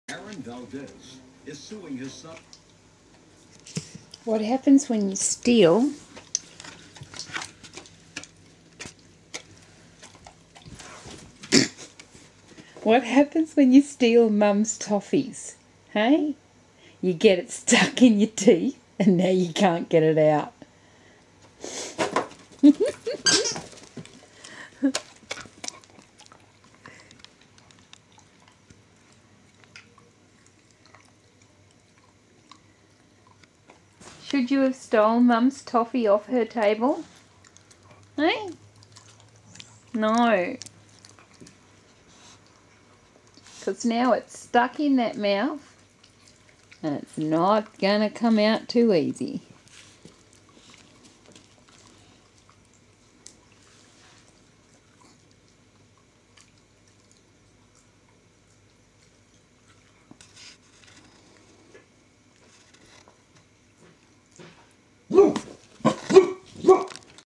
Custard busted stealing Toffees
Punishment for stealing toffees, poor girl took her hours to settle down